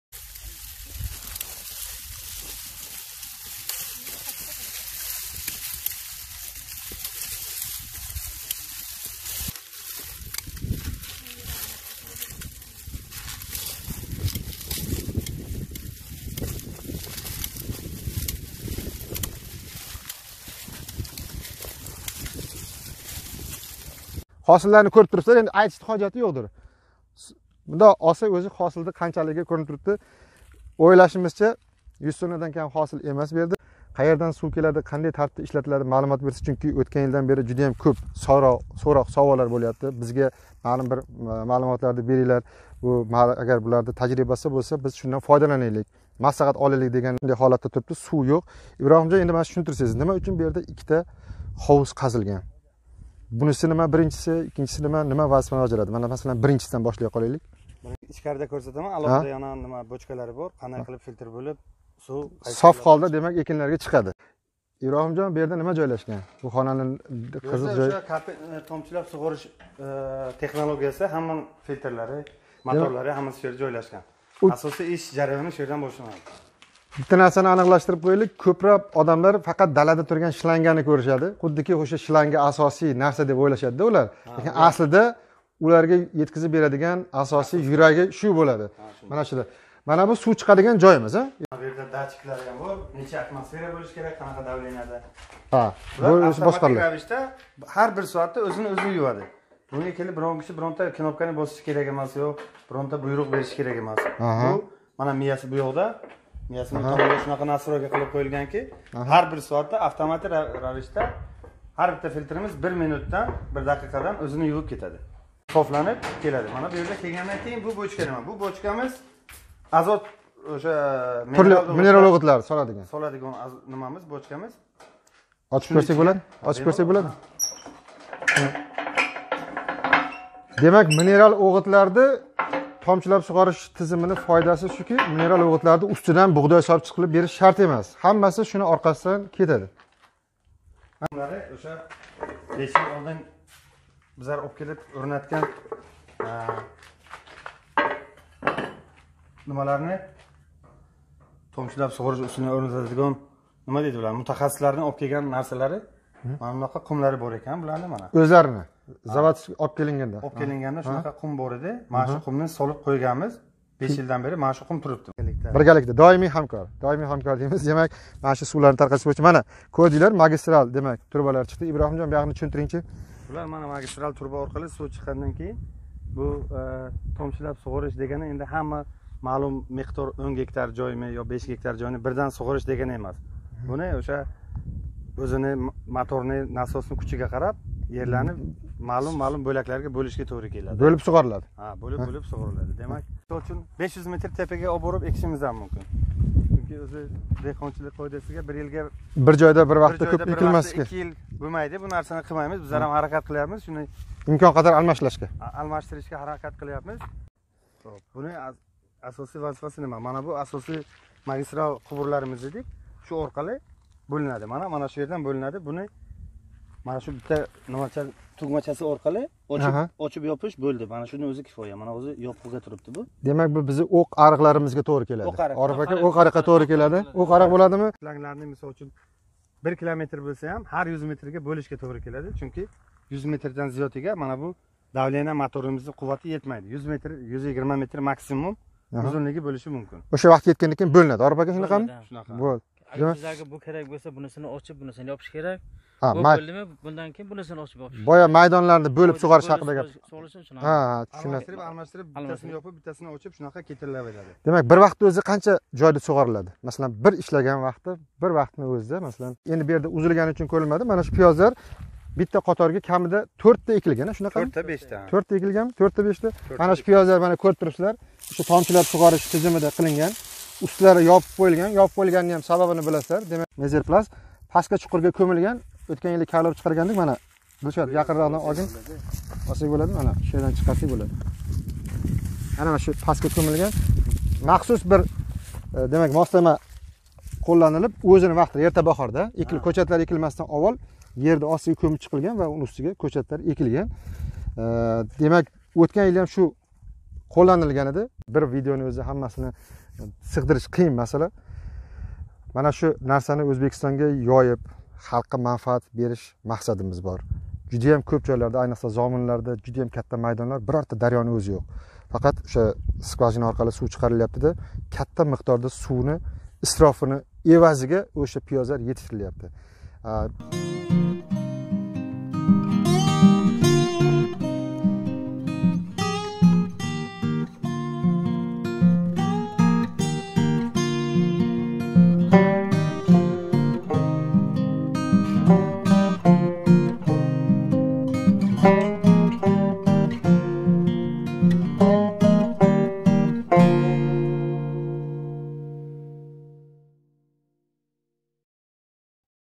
Пиёзни томчилаб суғориш 2019
Пиёз  экиш, етиштириш агротехникасида томчилаб суғориш (капельный полив) тизимини қўллаш хақидаги
маълумотлар кичик видеоформатда.  21- Сентябр 2019 йил. Самарқанд вилояти, Самарқанд тумани. «МАМАТ АГРО МЕВА» МЧЖ экин майдонларида. АНОНС...